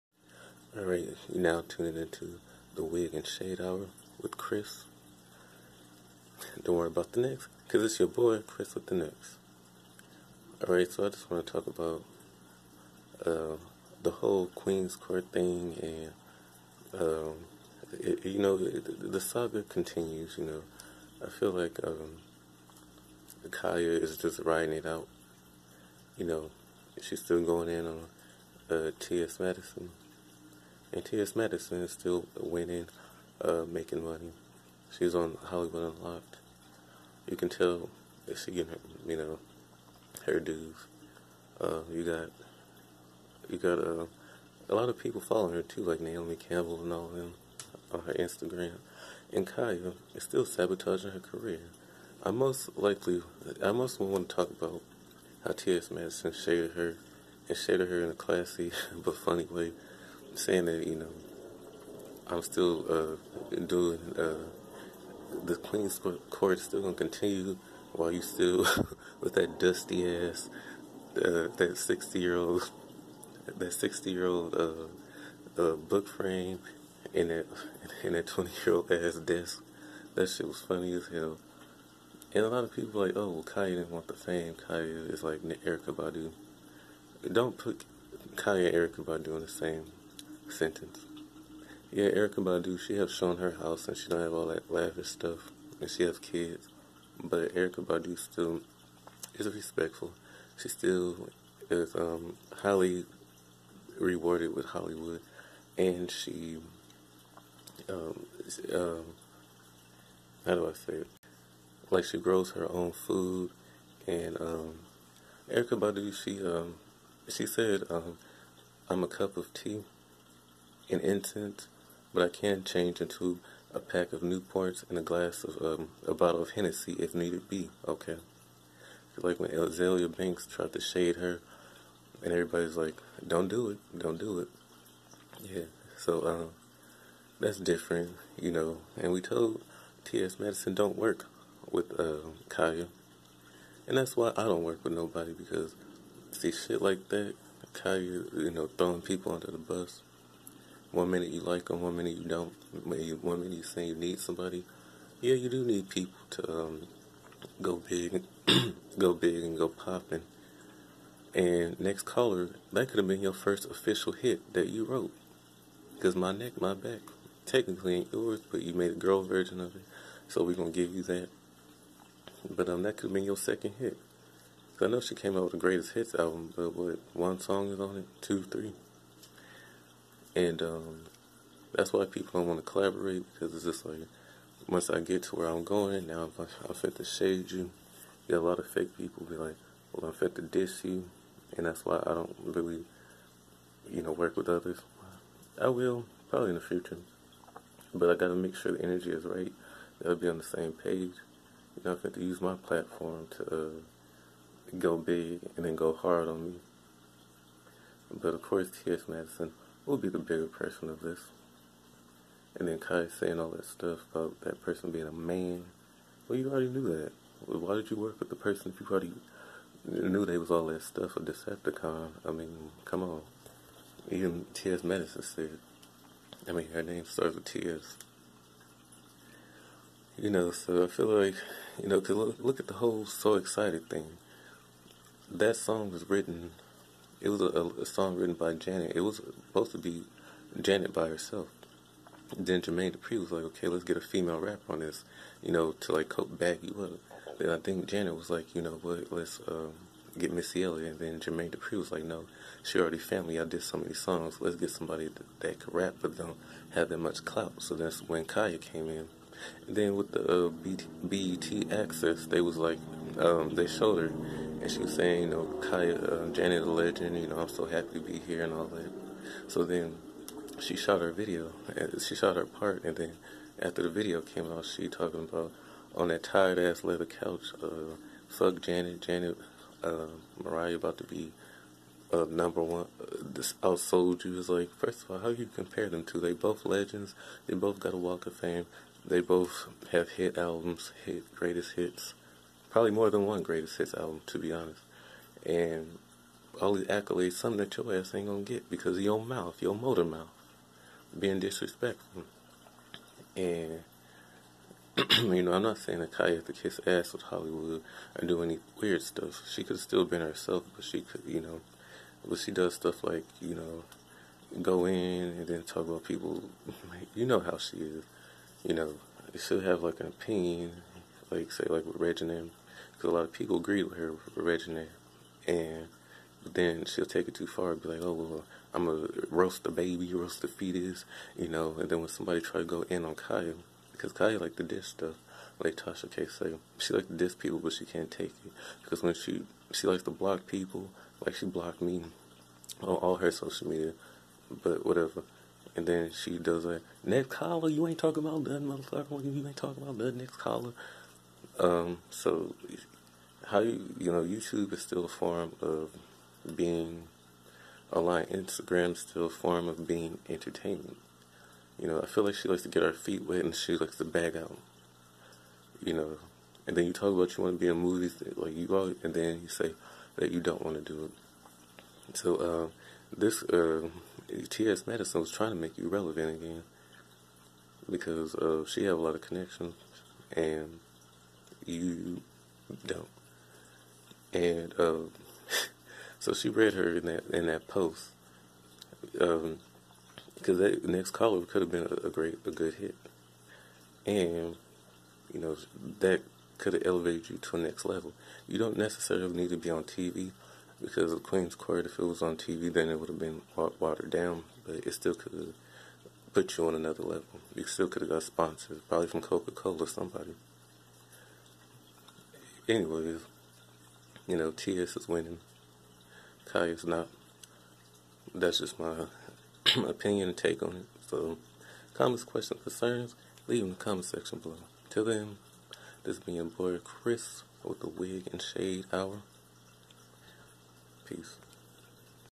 Ts Madison "VS" Khia! | XhriswithanX
Here's my take on the whole situation